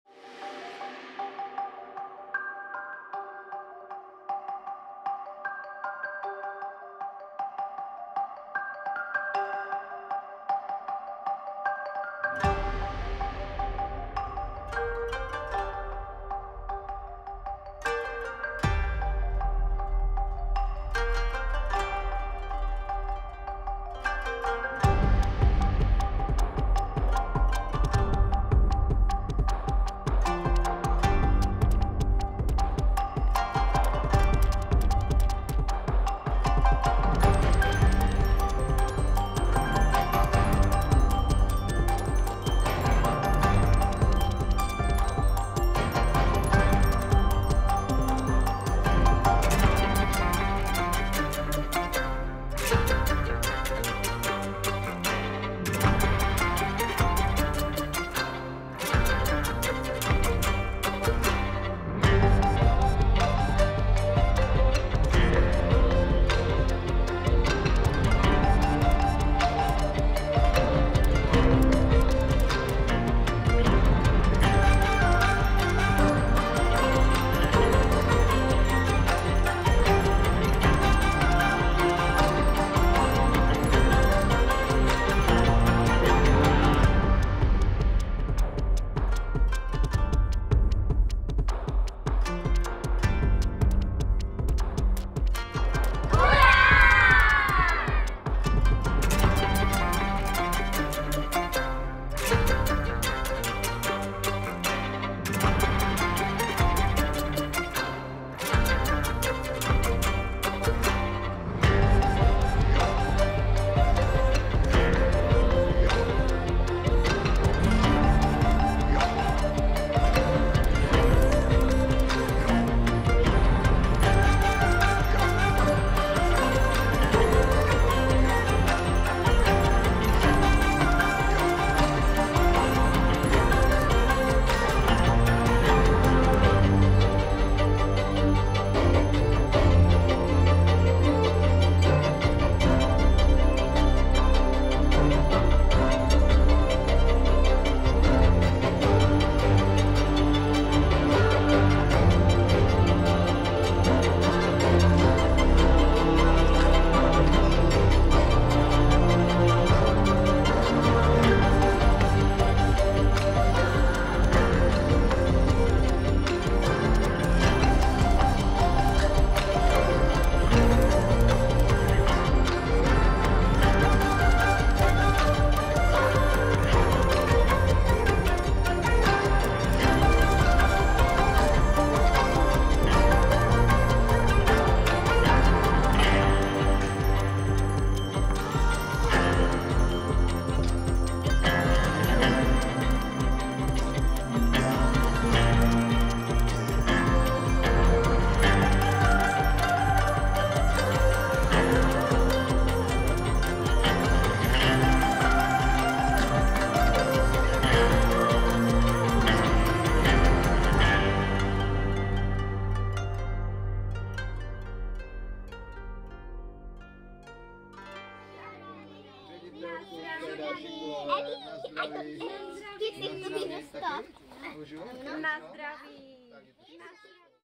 Pasování předškoláků 2021
Již podruhé jsme zavítali s kamerou do Mateřské školy v Želatovicích, kde probíhalo loučení s předškoláky. Nechybělo spousta soutěží, pořádná královská hostina a na konec i příchod samotného krále.